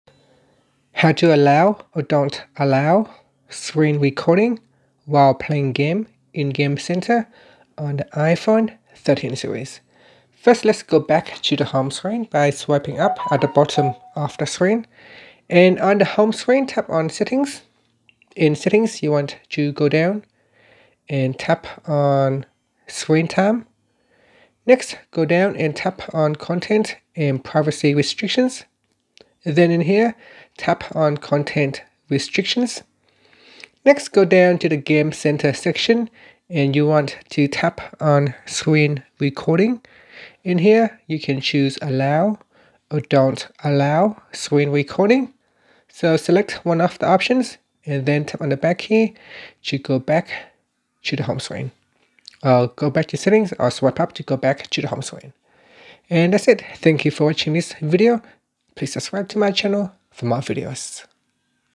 iPhone 13/13 Pro: How to Allow/Don't Allow Screen Recording While Playing Games in Game Center
Learn how you can allow or don't allow screen recording while playing games in the Game Center on the iPhone 13 / iPhone 13 Pro.